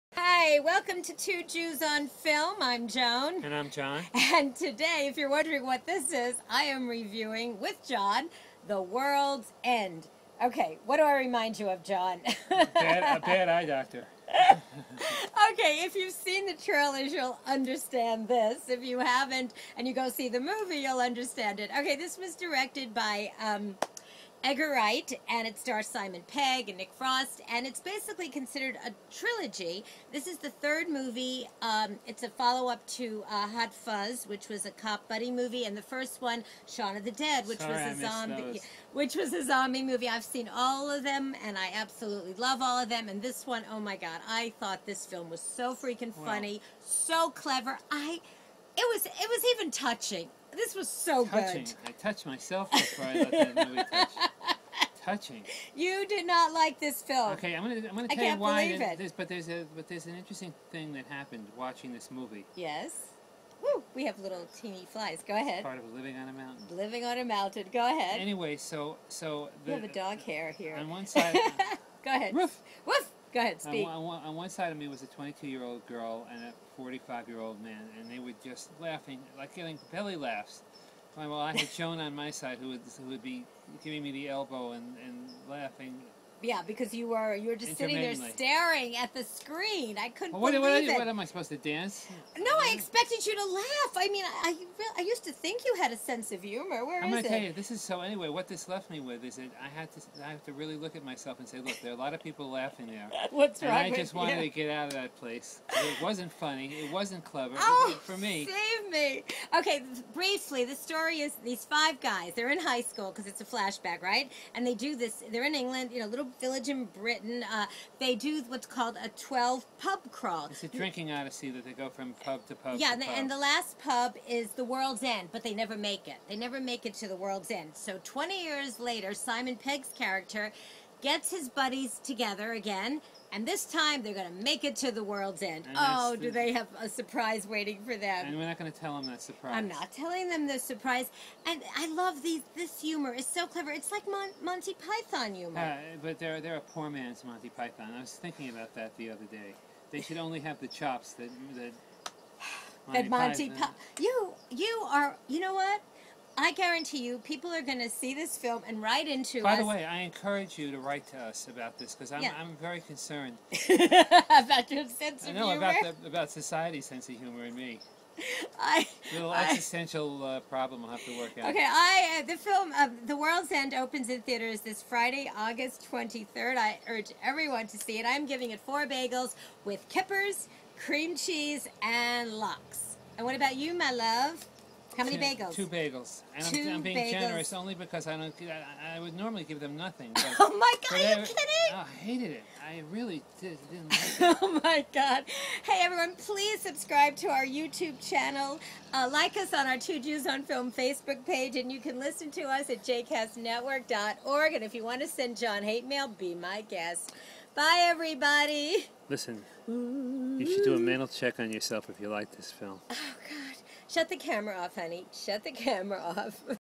Two Jews On The World's End
'The World's End Movie Review
By Joan Alperin Schwartz
We're all familiar with the quote, 'You can never go home again' .  Well, that's not exactly true...You can go home again, but it may not be the way you remembered it.

That is certainly the case in 'The World's End' directed by Edgar Wright.  Wright wrote the script with Simon Pegg, who also stars in the film.

The writers  consider this one to be the last in a trilogy...A follow up to,...'Shaun of the Dead', the zombie movie and the cop buddy pic, 'Hot Fuzz'.

The film starts out with a flashback.  The year is 1990 and five high school friends, living in the English village of Newton Haven, attempt a 12 pub crawl.  Unfortunately, they never quite make it, stopping before they reach the last one...The World's End.

Flash forward 20 years later, Gary (Simon Pegg) contacts his former mates...Andy (Nick Frost) who he's been estranged from...Steven (Paddy Considine)...Oliver (Martin Freeman) and Peter (Eddie Marsan) to once again reunite and this time...Make it all the way to the end...The World's End.

Of course, Gary's mates have changed.  They are all family men with careers.  Gary however, is the same lost dude he was in high school.  So for him, this 12 pub crawl means everything.

What starts out as a reunion, quickly turns into something much different and the guys soon realize, that the fate of humanity is in their hands.

'The World's End' which opens in theatres, Friday August 23, 2013 is not only a very funny movie, but it's also clever and full of surprises.

In the end, it's not only a story about how far a man will go for a pint, but how far will he  go for his mates.

I gave 'The World's End' 4 1/2 bagels out of 5...John seems to have lost his sense of humor and gave it a much different score.

Check out our video to see what he thought and for more of our rantings.

Let us know what you think about 'The World's End'.  Thanks everyone